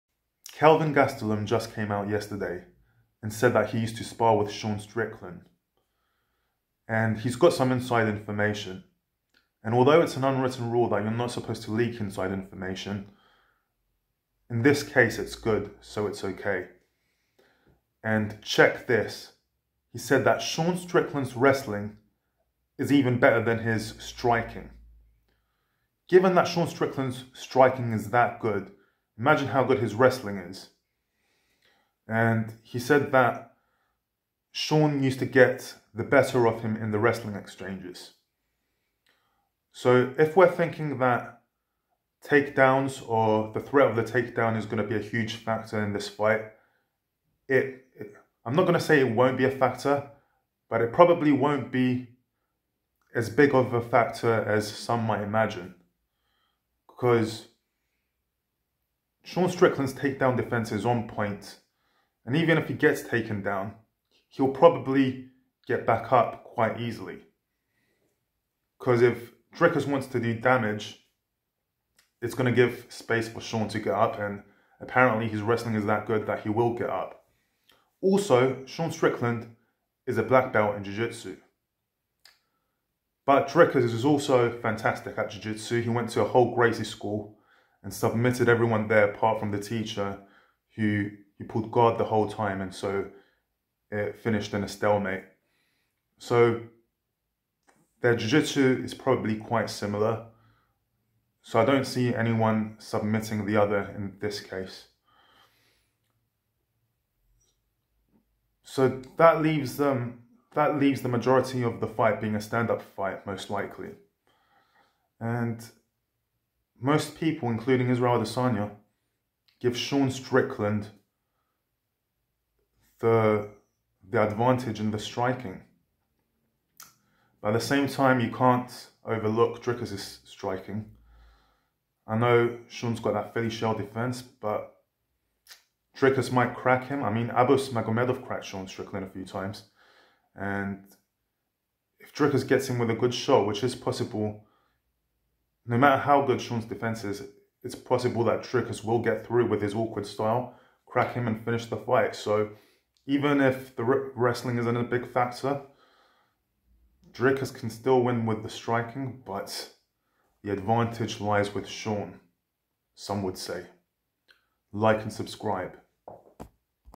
Sean Strickland better wrestler than Dricus... cap? Kelvin Gastelum says otherwise
Kelvin Gastelum said that Sean Strickland's wrestling is even better than his striking. However how would he compare to Dricus Du Plesis